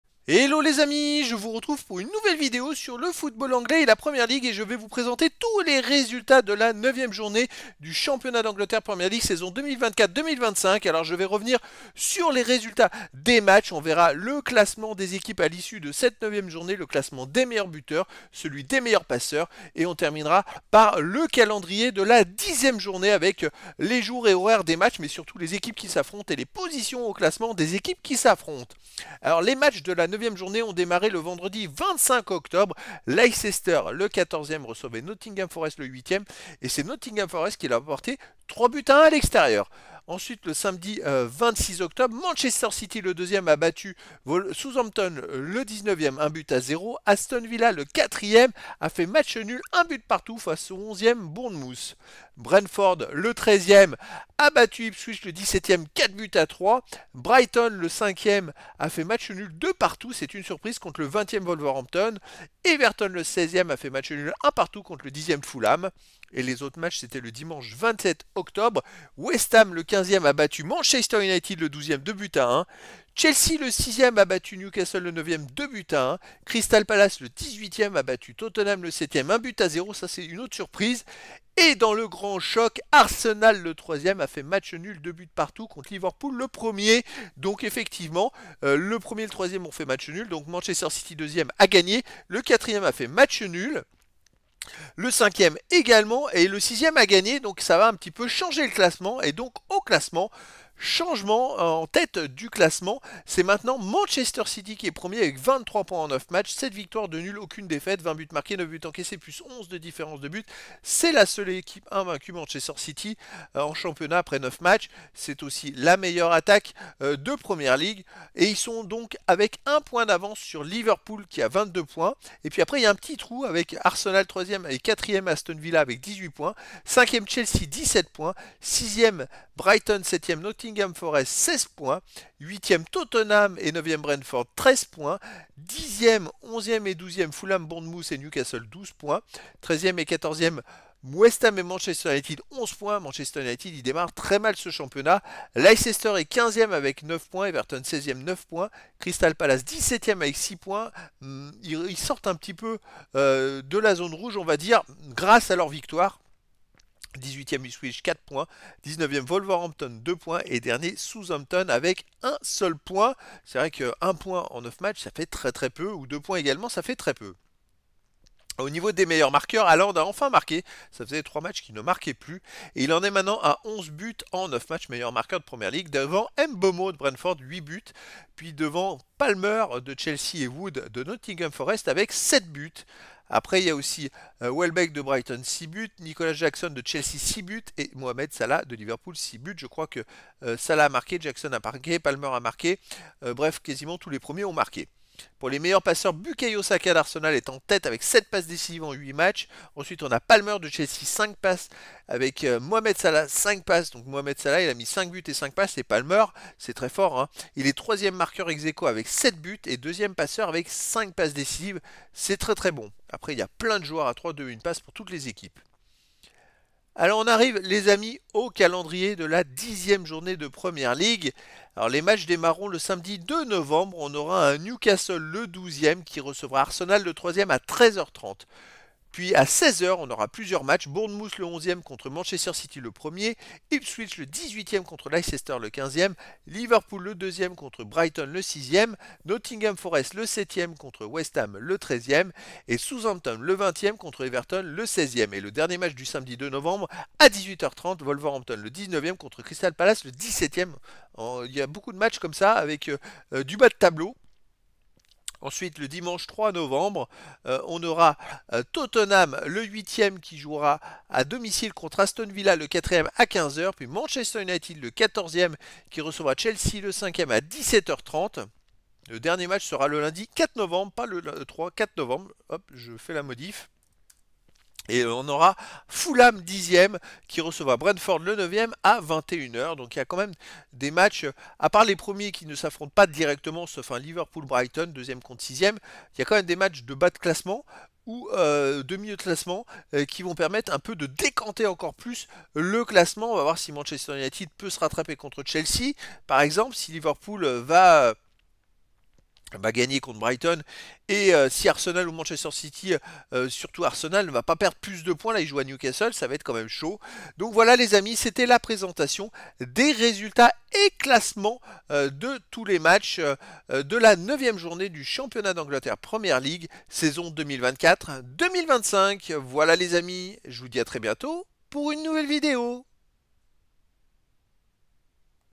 PREMIER LEAGUE - 9ème journée 2024/25 : Résultats - Classements - Buteurs - Passeurs - Stats - Infos
Je vous présente les résultats des matchs de football de la 9ème journée du championnat d'Angleterre de Premier League, saison 2024/2025.

Je reviens sur tous les matchs avec les scores et de nombreuses informations et statistiques.

Puis je vous présente le classement de la Premier League à l'issue de cette journée.

Je vous propose ensuite le classement des meilleurs marqueurs puis le classement des meilleurs passeurs décisifs de Premier League.

Et je termine sur le calendrier de la prochaine journée de championnat avec les matchs à venir et les horaires.

Voici les résultats de la 9ème journée :

Leicester - Nottingham Forest : 1 - 3

Aston Villa - Bournemouth : 1 - 1
Brentford - Ipswich : 4 - 3
Brighton - Wolverhampton : 2 - 2
Everton - Fulham : 1 - 1

Crystal Palace - Tottenham : 1 - 0